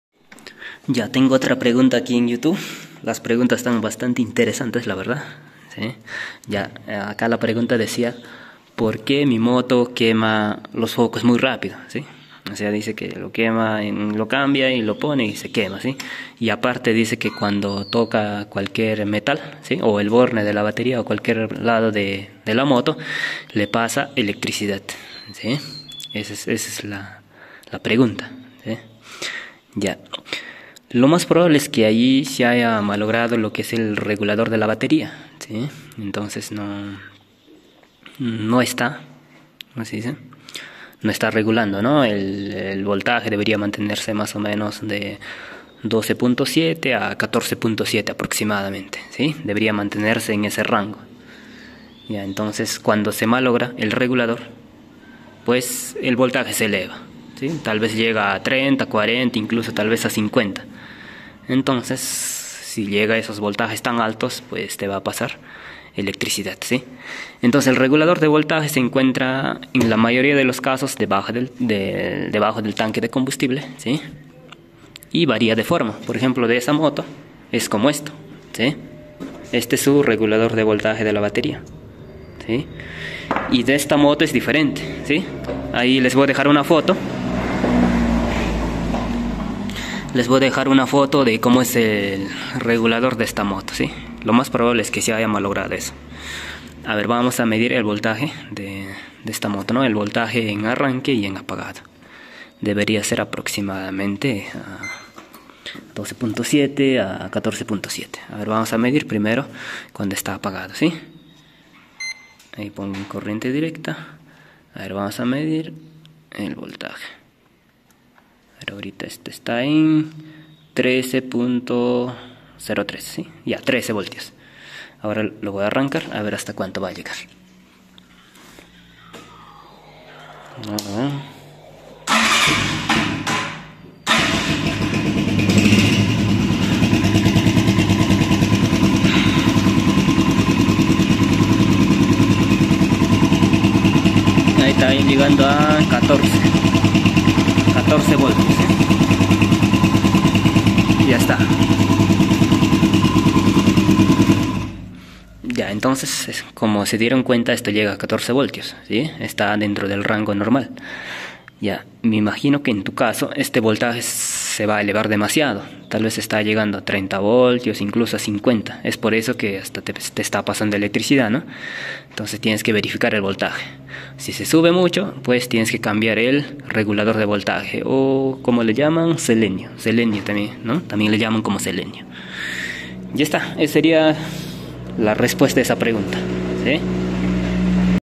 Se queman los focos y pasa electricidad en la moto.
En este video se explica la razón por el cual se queman los focos muy rápido y pasa electricidad en una motocicleta.